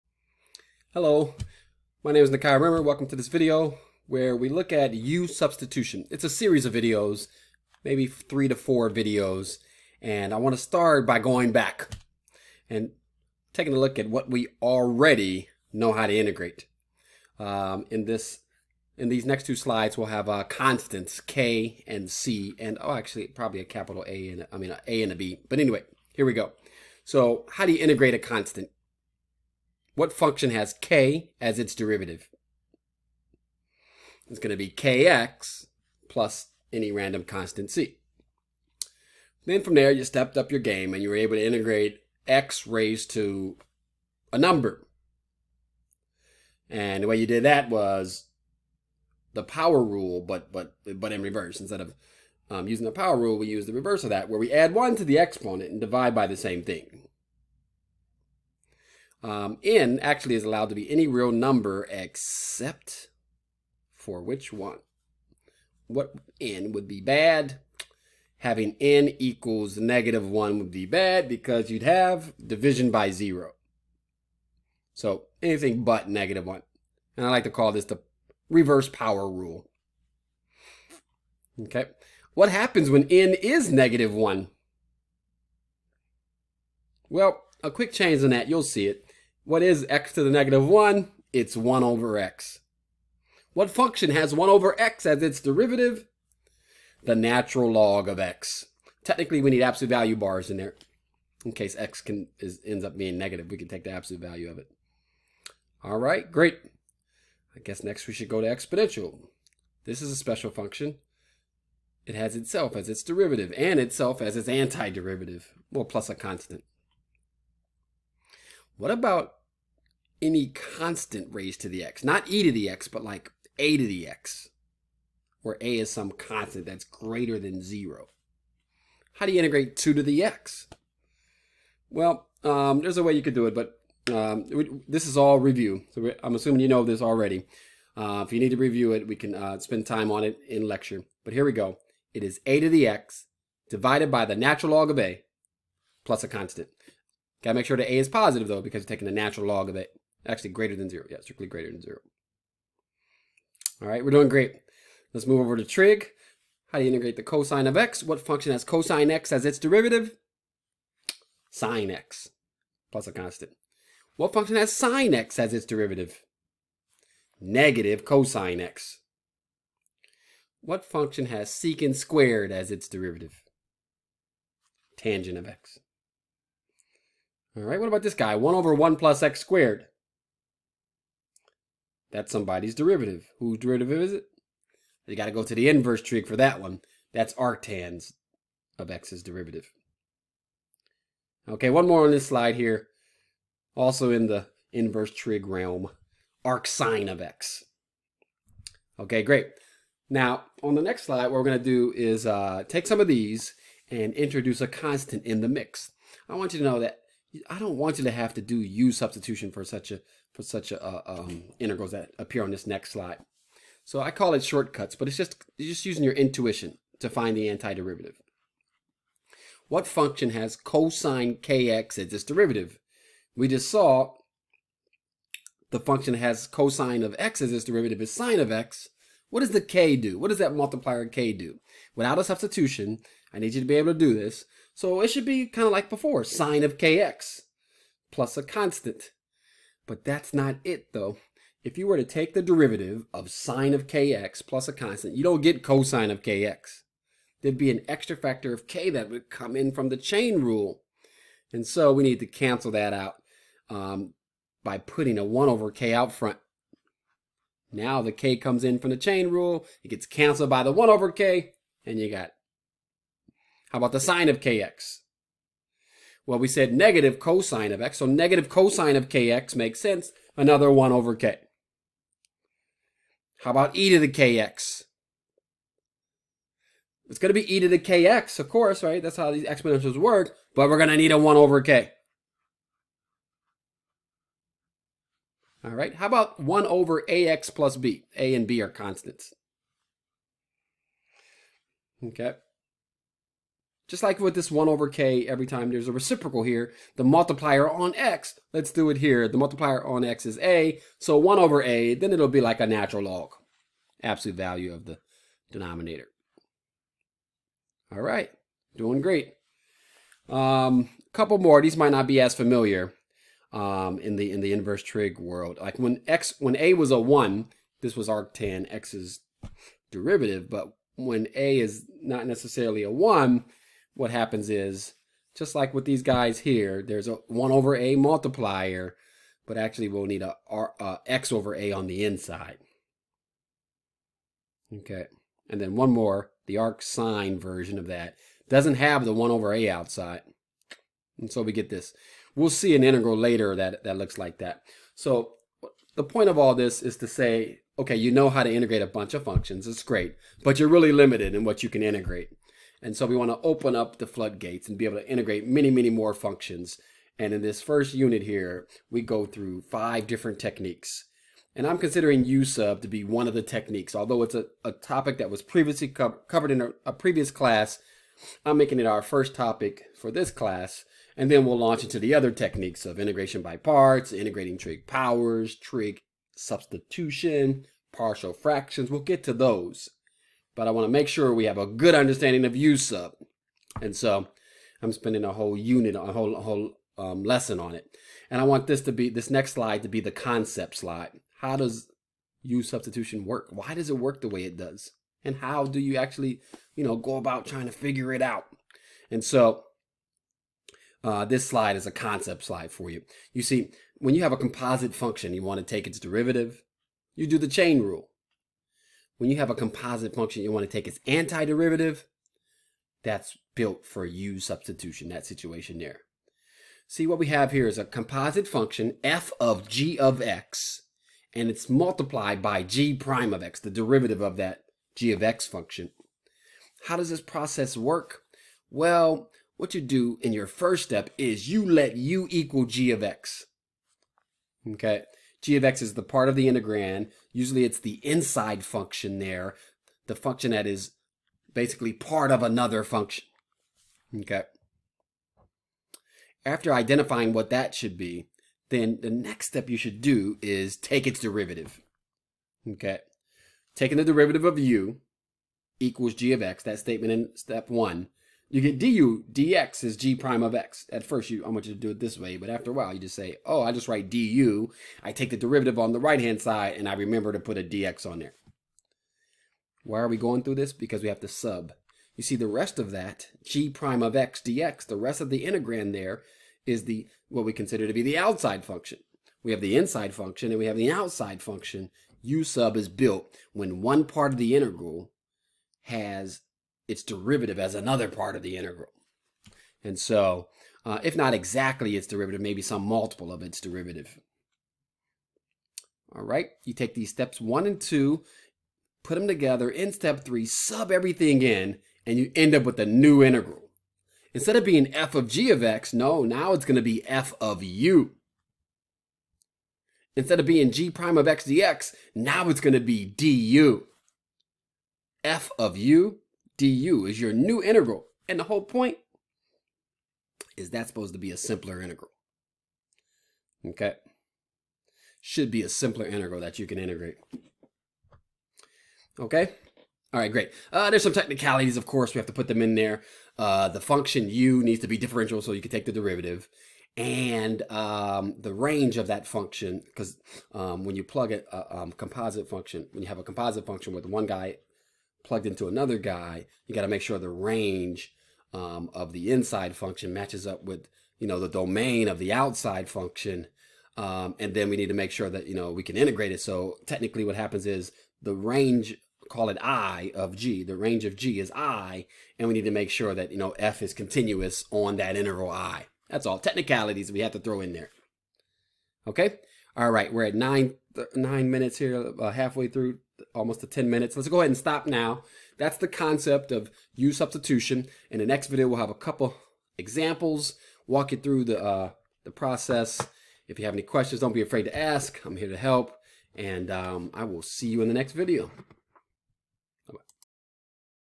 U-Substitution Concept Video and Review of Basic Integrals
In this video, we start with a review of basic integration and then we discuss the concept of u-substitution.